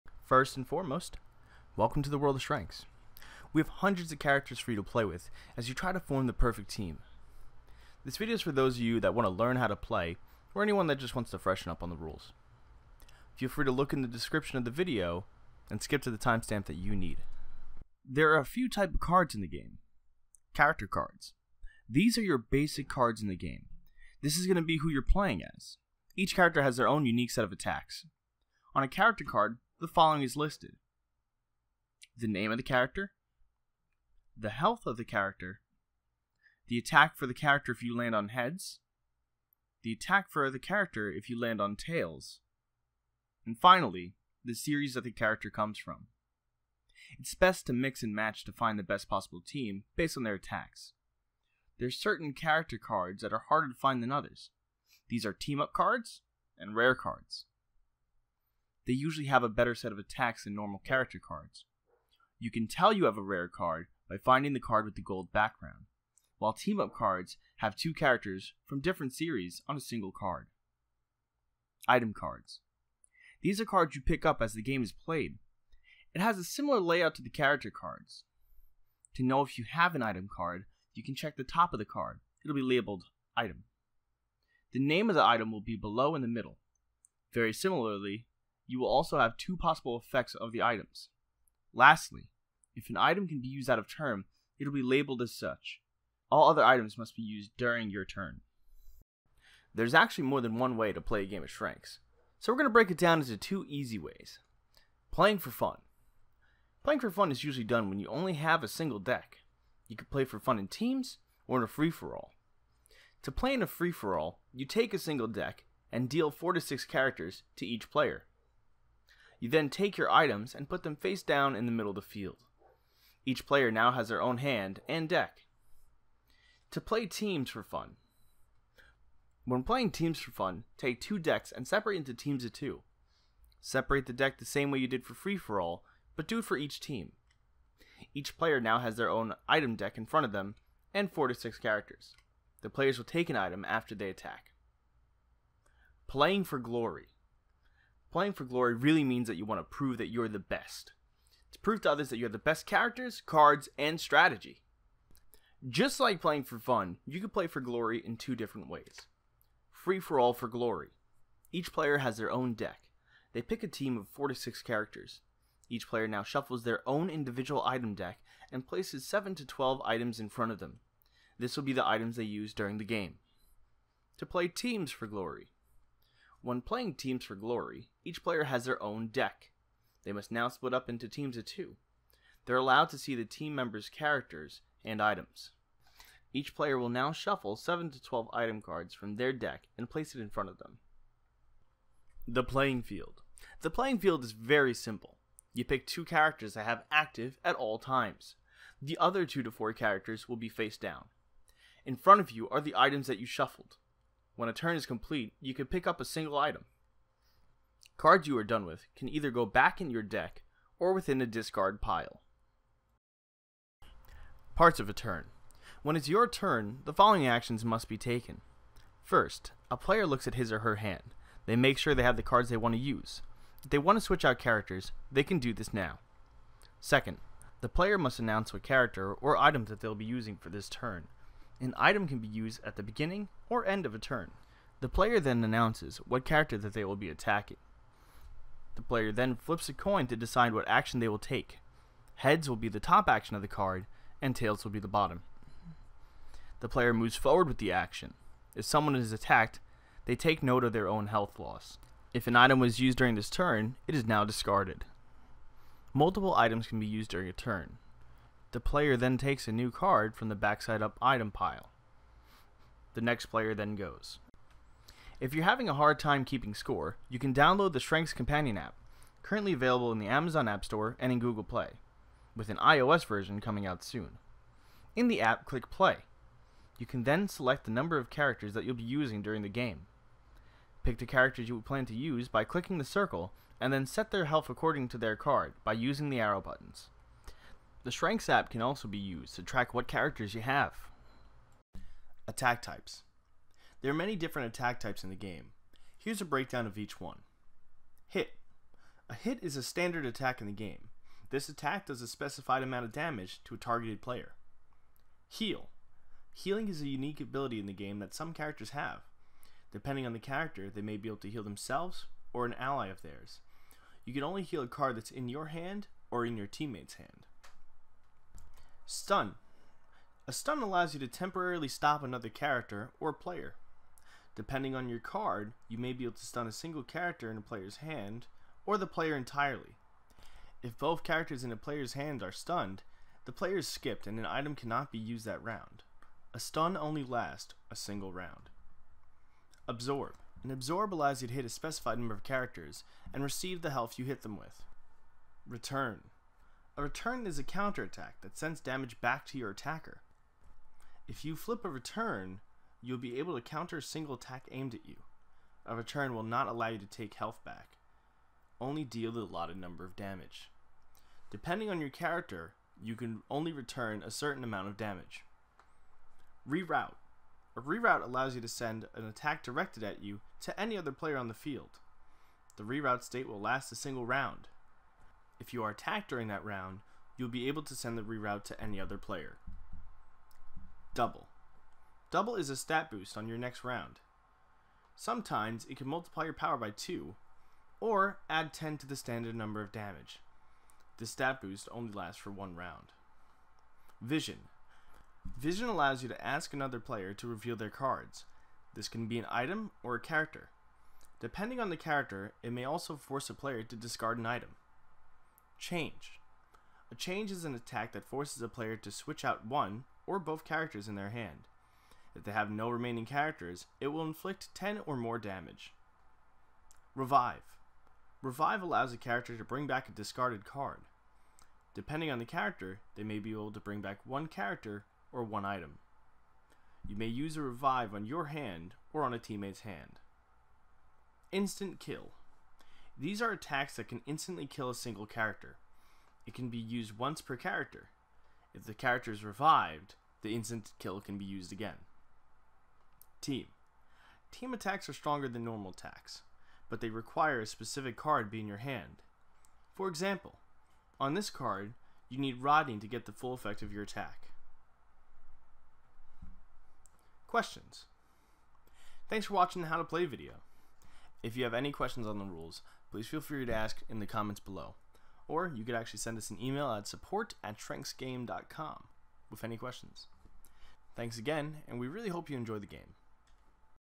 How to Play Shranks - Save the Multiverse!
Learn how to play the party/trading card game to challenge your friends and save the multiverse!

Sections:

Character Cards - 0:20
Item Cards - 1:12
Ways to Play - 1:39
       Playing for Fun - Free for All - 1:44
       Playing Teams for Fun - 2:05
       Playing for Glory - Free for All - 2:25
       Playing for Glory - Teams for Glory - 2:54
The Playing Field - 3:12
Parts of a Turn - 3:34
Shranks App - 4:24
Attack Types - 4:52
            Hit - 4:57
            Heal - 5:04
            Stun - 5:17
            Absorb - 5:39
            Return - 5:46
            Reroute - 6:06
            Double - 6:21
            Vision - 6:33
            Change - 6:45
            Revive - 6:56
            Instant Kill - 7:11
            Team - 7:22
Questions - 7:34